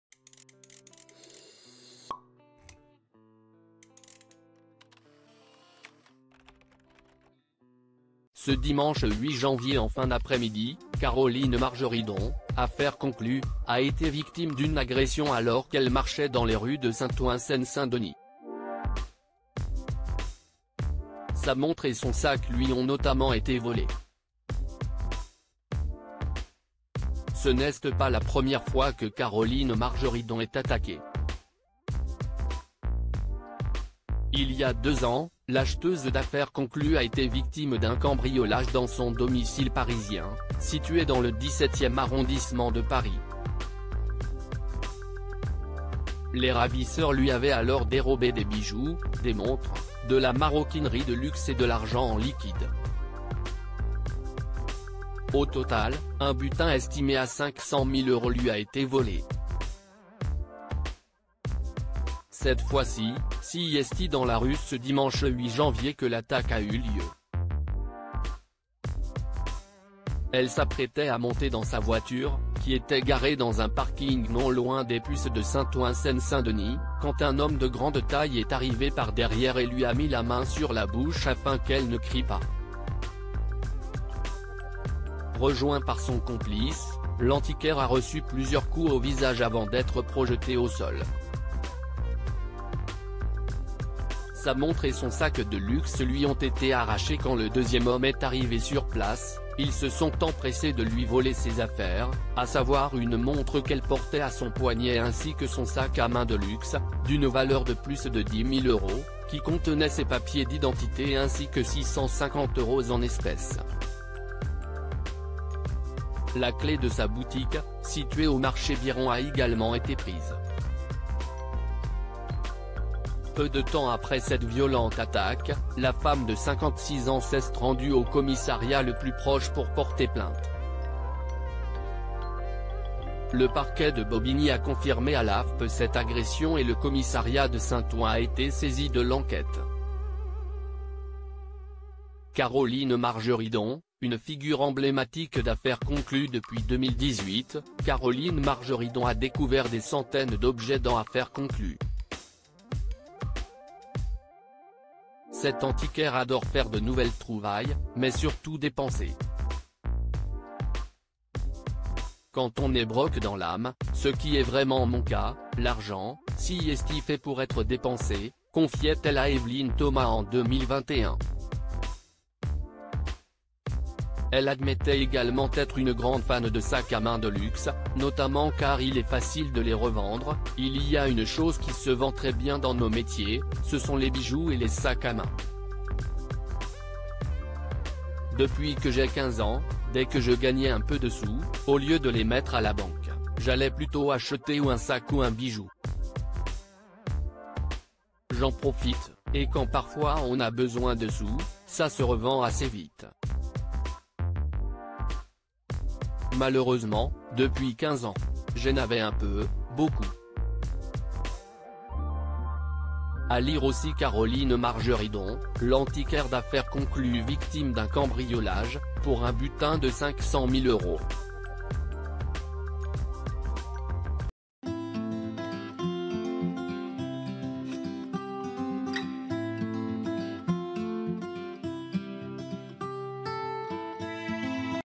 Caroline Margeridon (Affaire Conclue) victime d'une violente agression et dépouillée en région paris
Caroline Margeridon (Affaire Conclue) victime d'une violente agression et dépouillée en région parisienne